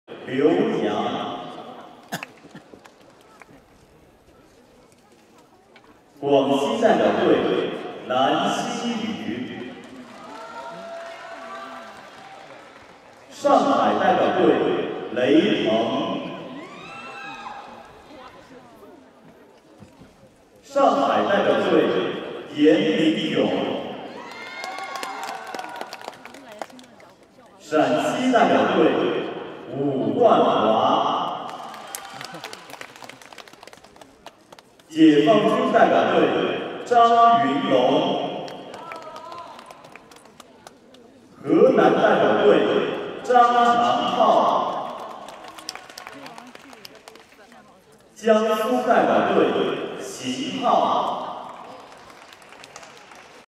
MAG SR EF intro, 2017 CHN Nationals, Wuhan
Liu Yang (Army), Lan Xingyu (Guangxi), Lei Peng (Shanghai), Yan Mingyong (Shanghai), Wu Guanhua (Shaanxi), Zhang Yunlong (Army), Zhang Changhao (Henan), Xin Hao (Jiangsu).